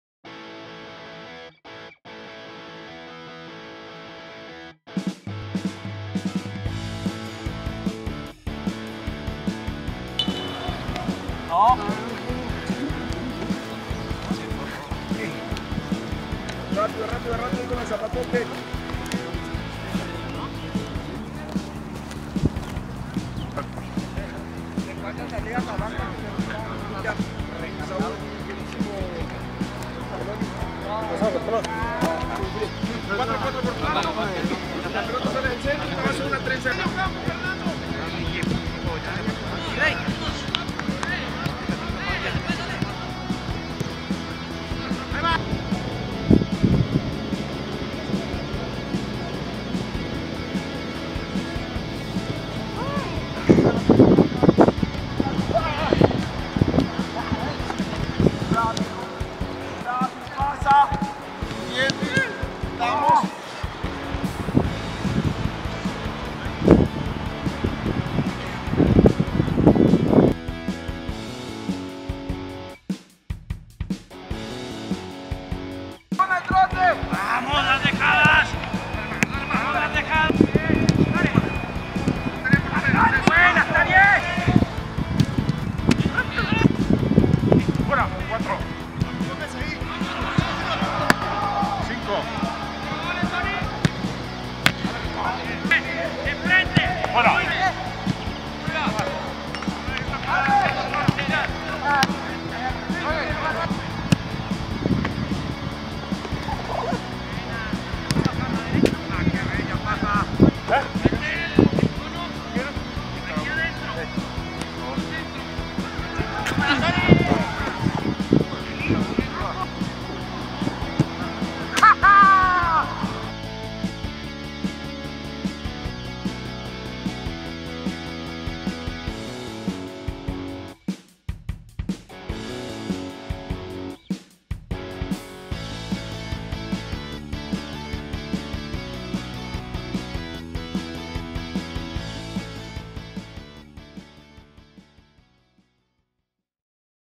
Trabajo de pretemporada /// Lunes 19 de junio
Te dejamos cómo se vivió el entrenamiento de este lunes sobre el Estadio Francisco Villa. Like, etiqueta y comparte :)

Te recordamos que este próximo miércoles a las once de la mañana tendremos un Facebook Live, prepara tus preguntas.